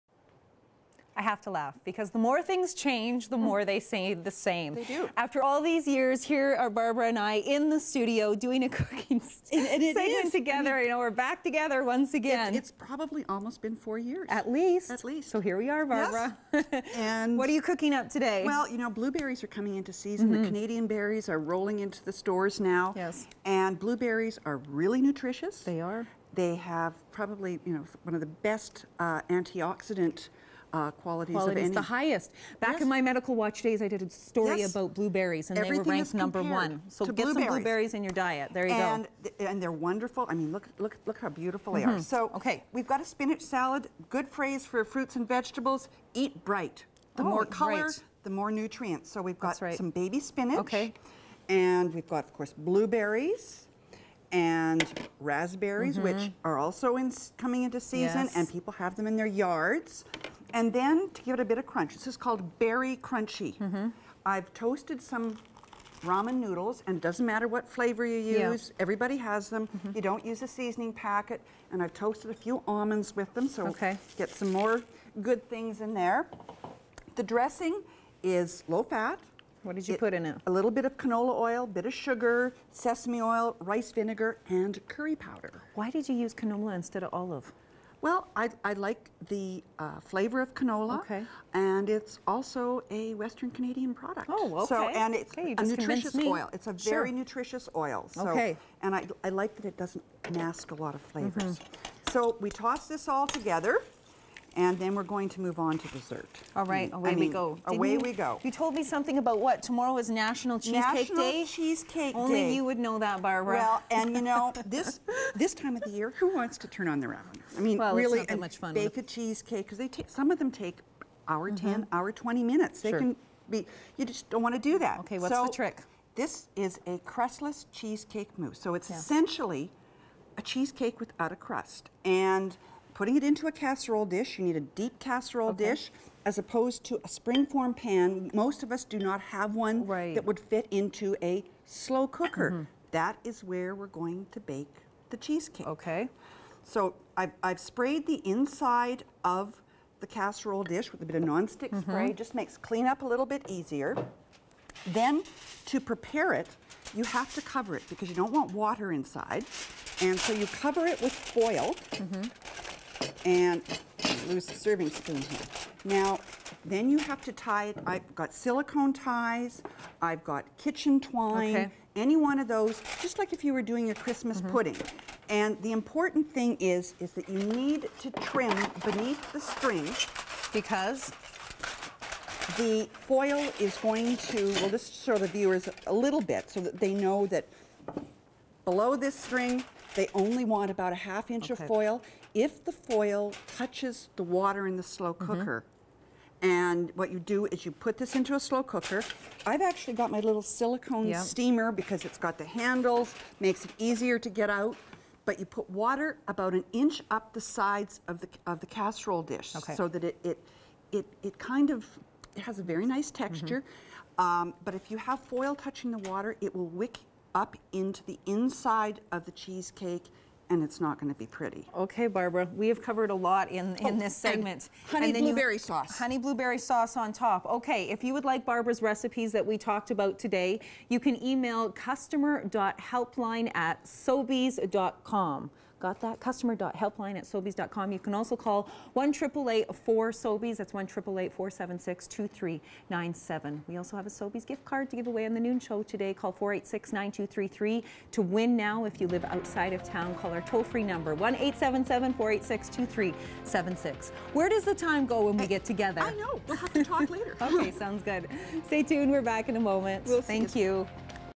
Barbara Barnes - CTV Edmonton (July 29, 2011)
Barbara Barnes stops by the CTV Studios to talk berries. blueberries to be exact.

Two great blueberry recipes are featured: Slow Cooker Cheesecake Mousse with Honeyed Blueberry Sauce and Berry Crunchy Spinach Salad.